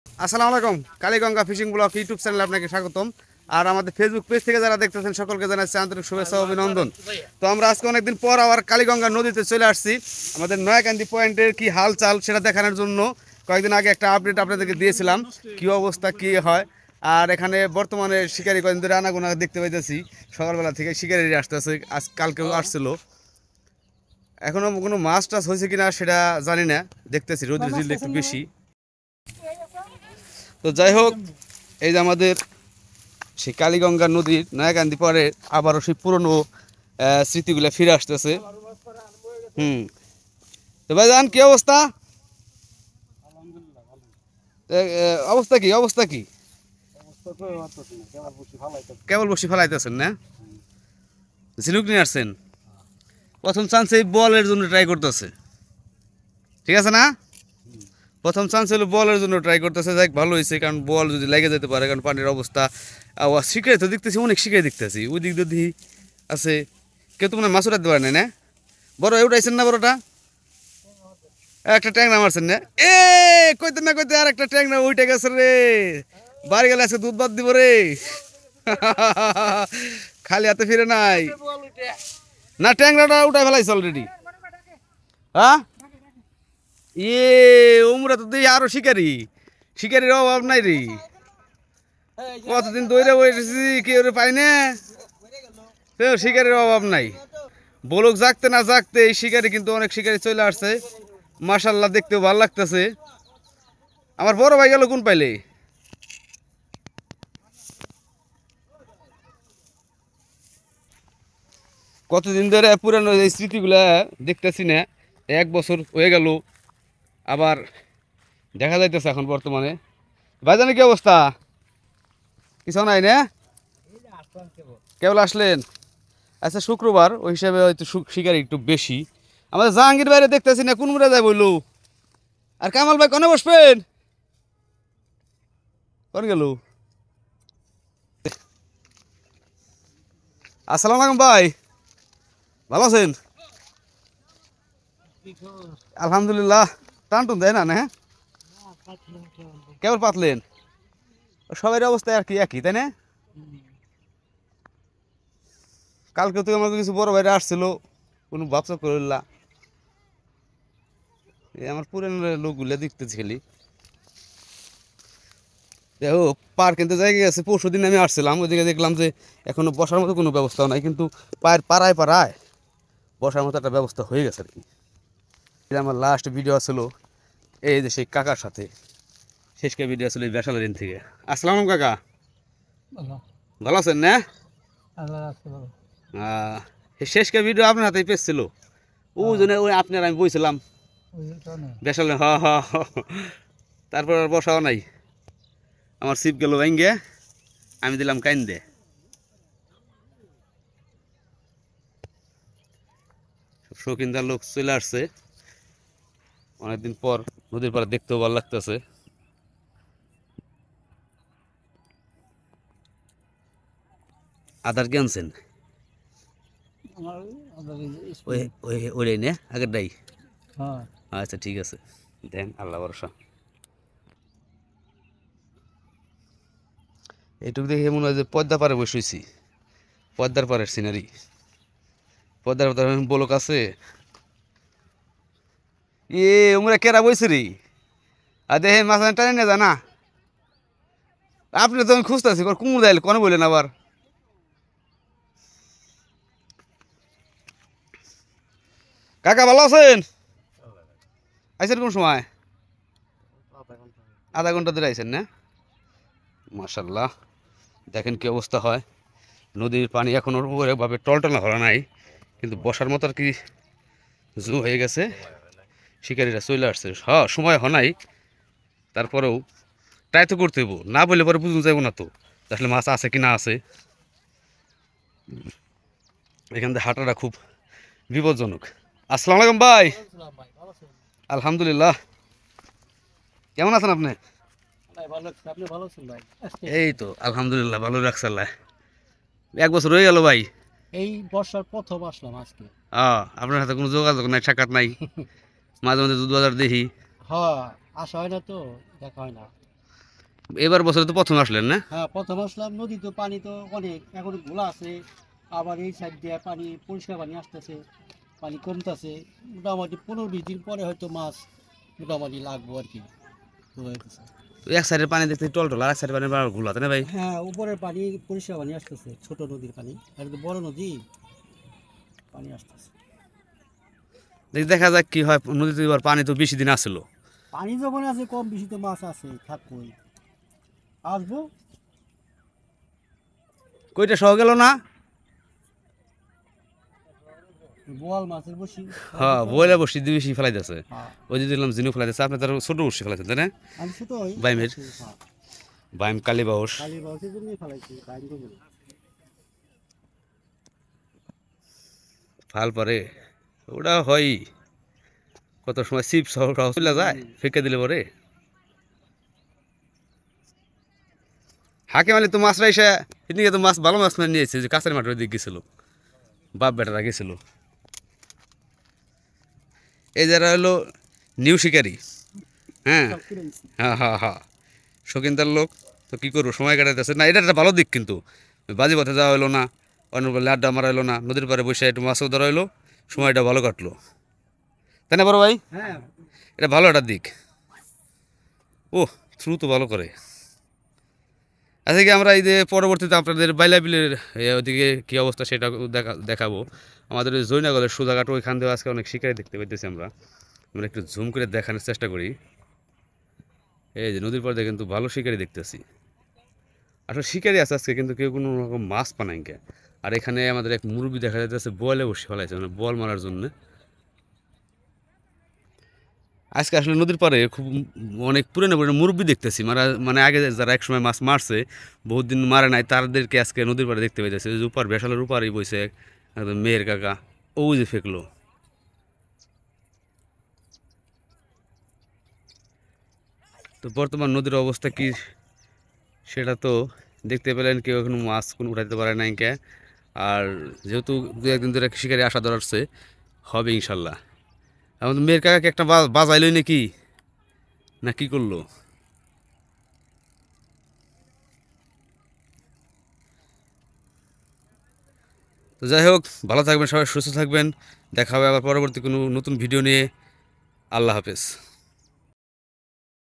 কালিগঙ্গা নদীতে মাছ শিকারীদের আনা গুনা শুরু।মানিকগঞ্জ নয়াকান্দি পয়েন্ট।
মাছ ধরা শুরু হয়েছে

Kaligonga fishing vlog ইউটিউব চ্যানেলে আপনাদেরকে স্বাগতম আমাদের সাথে থাকার জন্য আপনাকে অসংখ্য ধন্যবাদ।